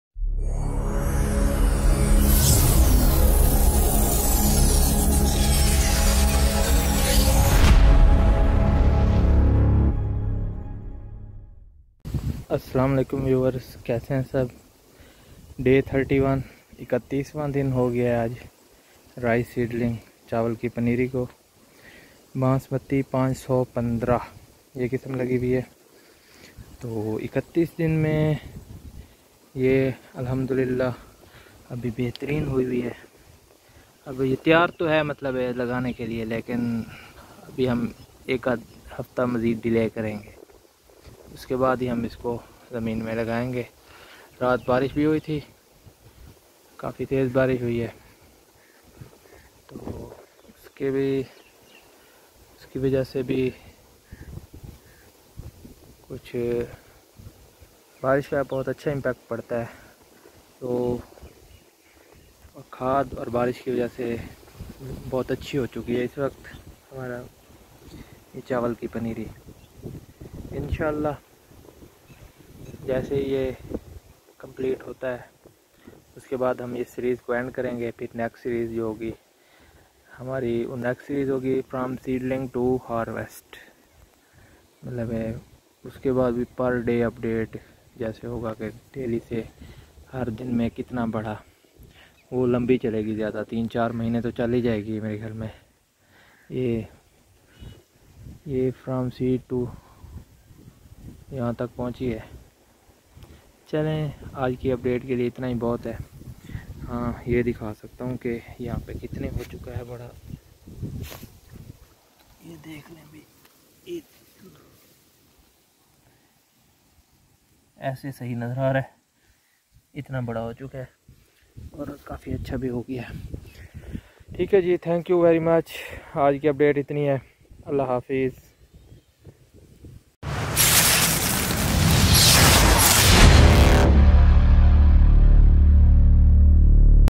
DAY 31 Rice Seedling 🌱|Dhan/Chawal Ki Paneri| @MansoorWorld96
Best quality rice Seedling DAY 31
Dhaan/Chawal ki paneri ka 31 din